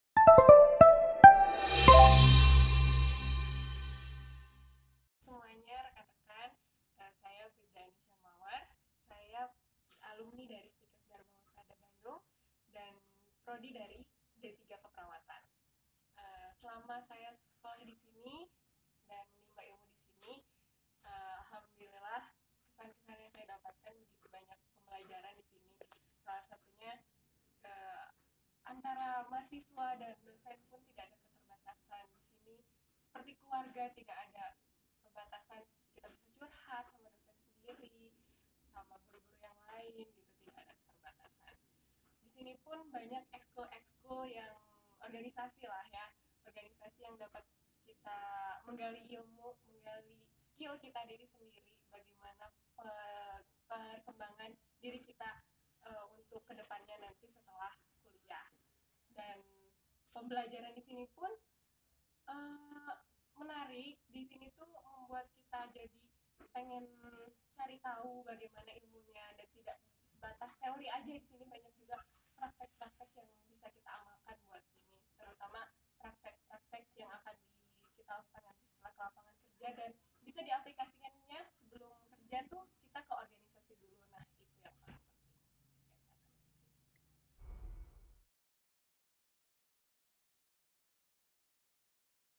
DHB Kata Alumni
STIKes Dharma Husada sebagai salah satu kampus terkemuka, telah menghasilkan ribuan alumni yang bekerja diberbagai segmen kehidupan mulai dari wirausaha, ASN, TNI, dll.
Anisyah ini merupakan salah satu alumni D3 Keperawatan yang telah menjadi anggota POLRI, menyatakan pengalaman selama menjadi mahasiswa DHB.